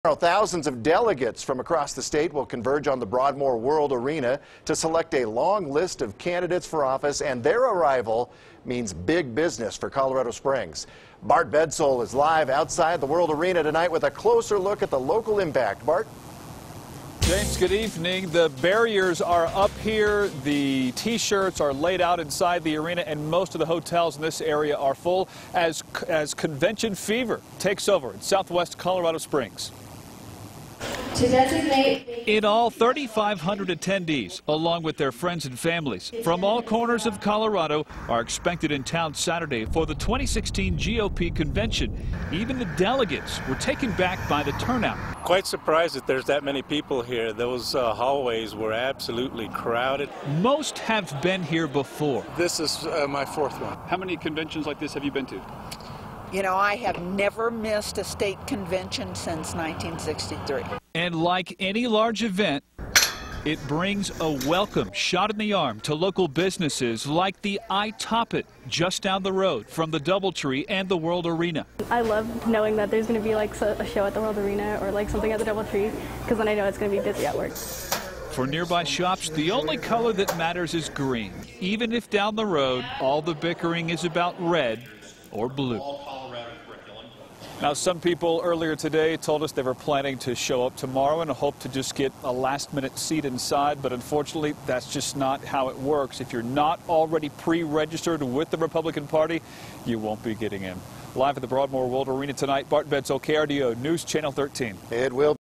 Convention fever hits Colorado Springs
Thousands of Colorado Republicans are expected to flood the World Arena area on Saturday for the state convention.